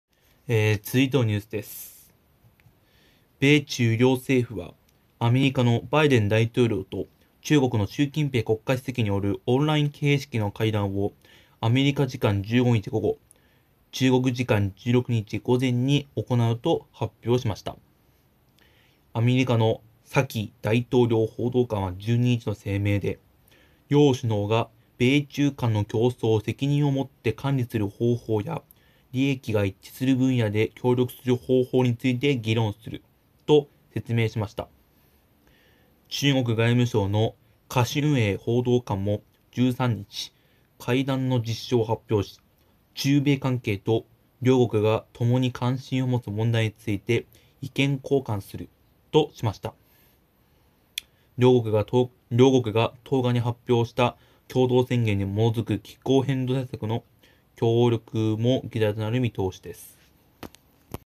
米中首脳会談 15日に行うことを発表
米中両政府はアメリカのバイデン大統領と中国の習近平国家主席によるオンライン形式の会談をアメリカ時間15日午後 (中国時間16日午前) に行うと発表しました。

アメリカのジェン・サキ大統領報道官は12日の声明で、両首脳が「米中間の競争を責任を持って管理する方法や利益が一致する分野で協力する方法について議論する」と説明しました。中国外務省の華春瑩報道官も13日、会談の実施を発表し、「中米関係と両国が共に関心を持つ問題について意見交換する」としました。両国が10日に発表した共同宣言に基づく気候変動対策の協力も議題となる見通しです。